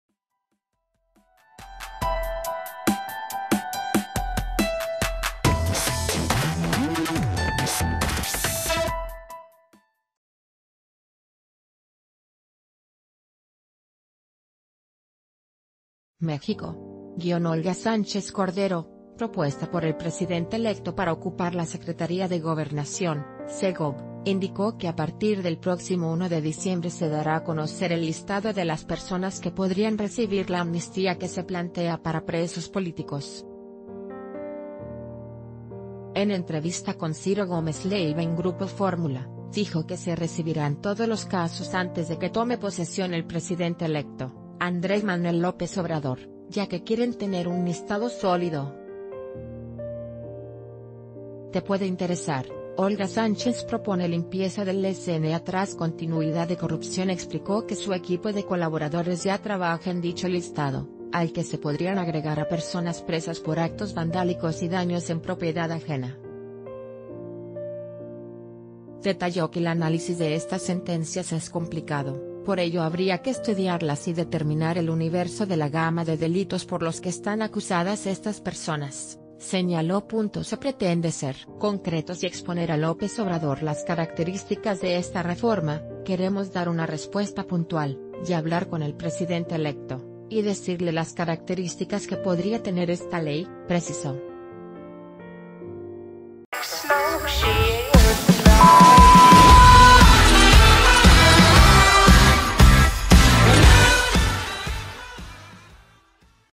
Anunciarán el 1 de diciembre la lista de personas que recibirían amnistía
México.- Olga Sánchez Cordero, propuesta por el presidente electo para ocupar la Secretaría de Gobernación (Segob), indicó que a partir del próximo 1 de diciembre se dará a conocer el listado de las personas que podrían recibir la amnistía que se plantea para presos políticos. En entrevista con Ciro...